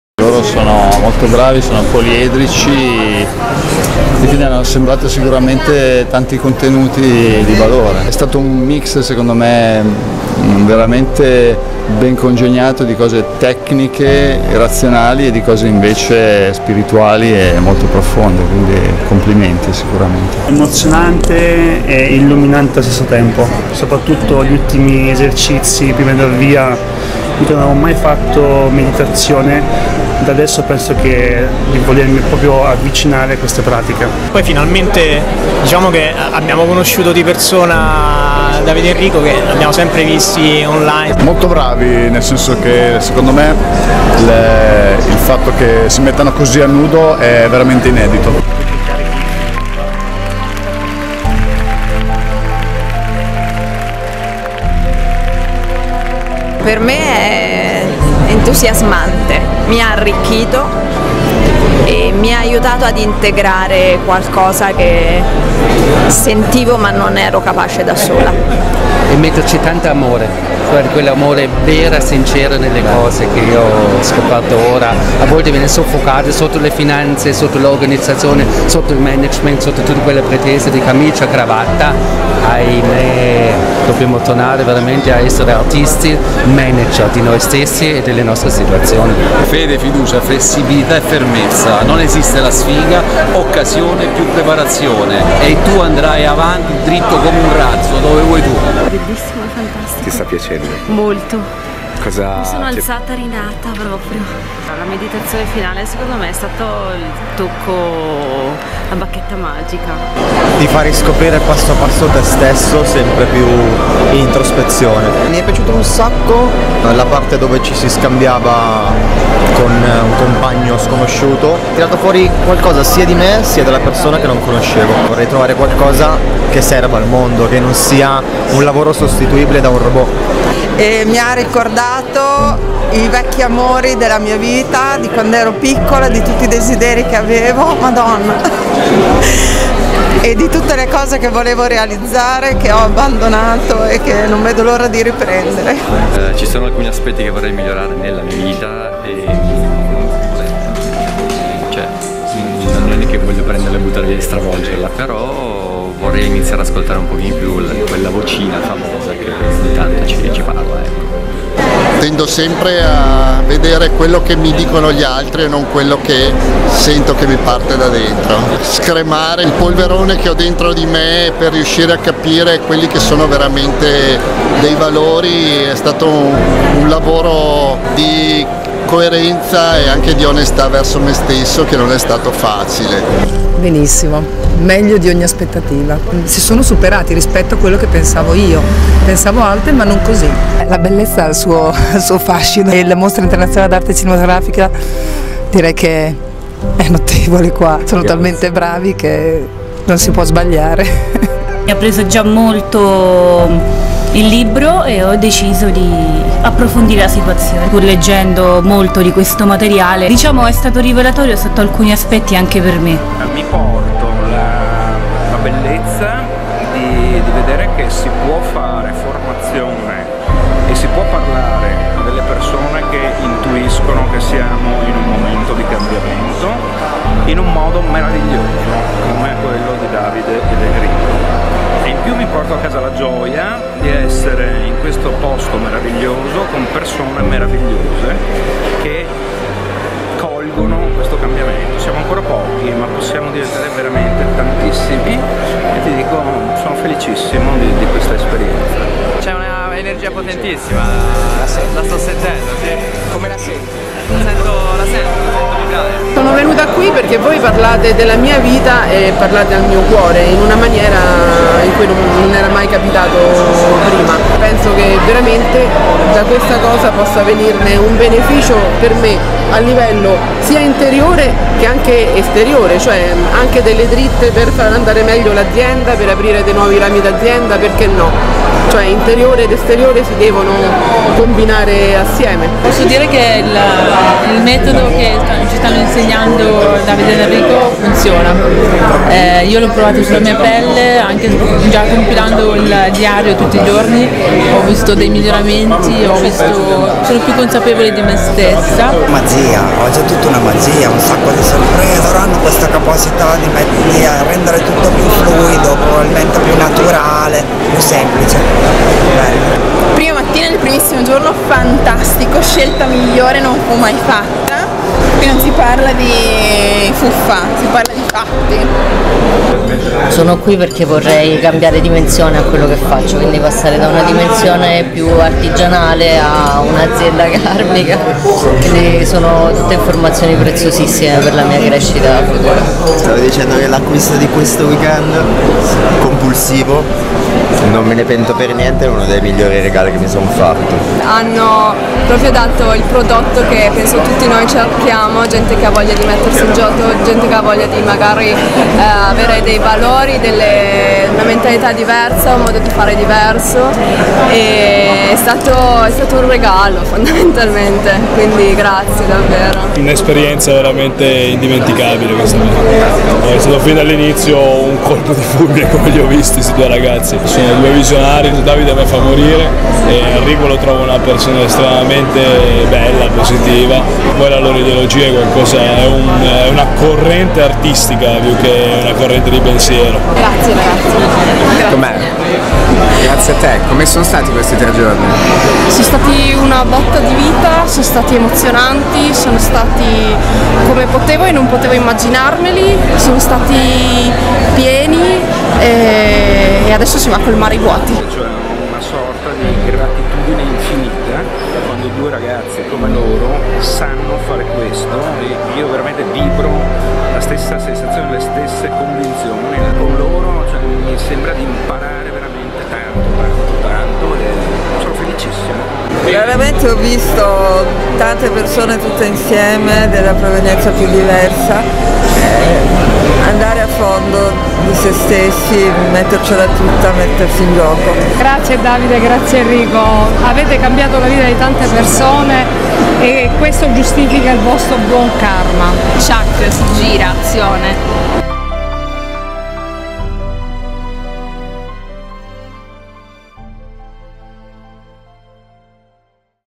RemakeYourLife 2018 - Cosa succede dopo che fai un corso di Life Design
Le voci di chi ha partecipato a Remake Your Life 2018, il primo corso europeo dedicato al Life Design che si è tenuto al Palazzo del Cinema di Venezia, tenuto dai MoneySurfers: Davide Francesco Sada ed Enrico Garzotto.